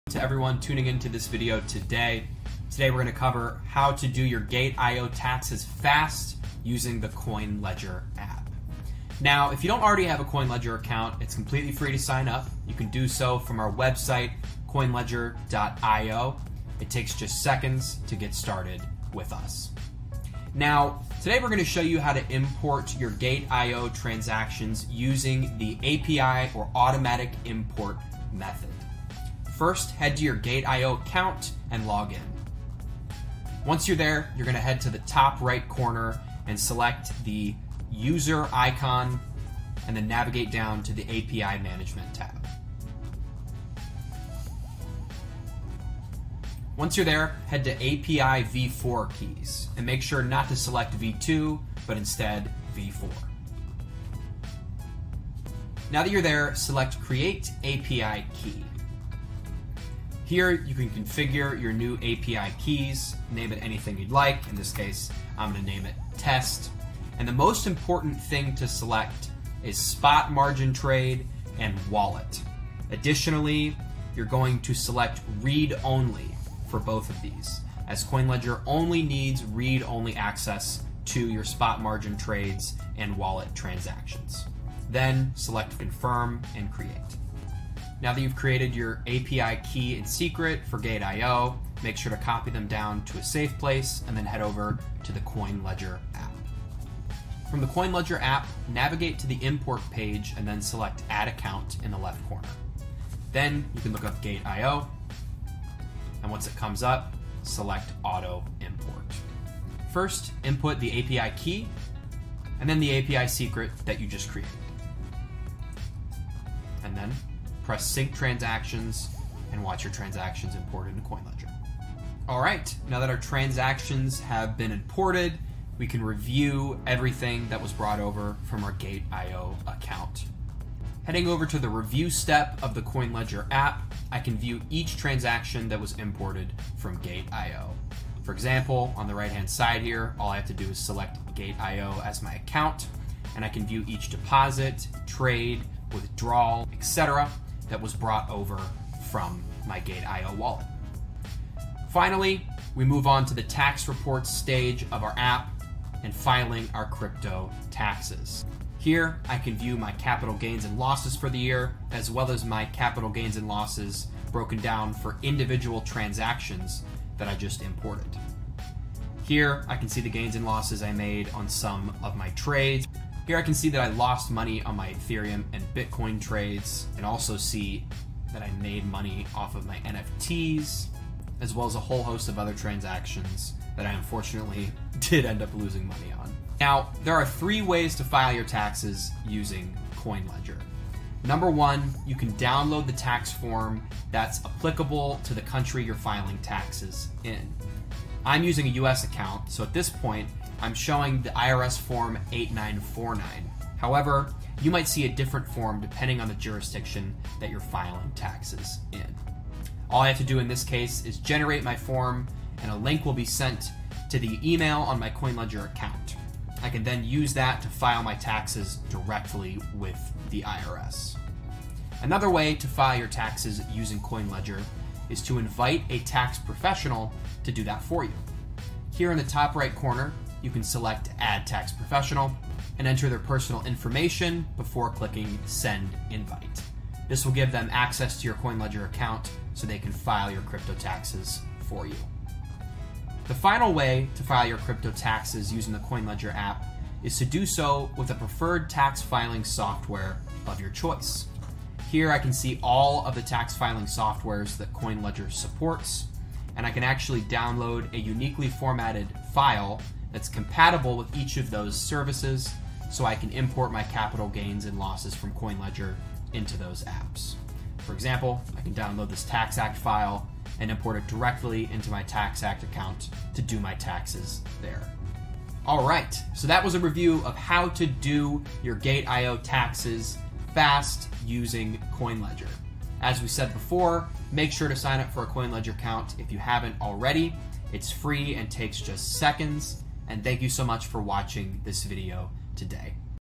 How to Calculate Your Taxes From Gate.io (The FAST Way!) | CoinLedger
Have you used Gate.io In this video, we walk through how you can use CoinLedger to calculate your Gate.io taxes fast and generate the necessary crypto tax forms you need.

Once you've used CoinLedger to generate your crypto tax forms, you can send them to your accountant, file them yourself, or import them into your preferred tax filing software!

Timestamps:

0:00 Introduction
0:20 How to Do Your Gate.io Taxes
2:31 Download your Gate.io tax forms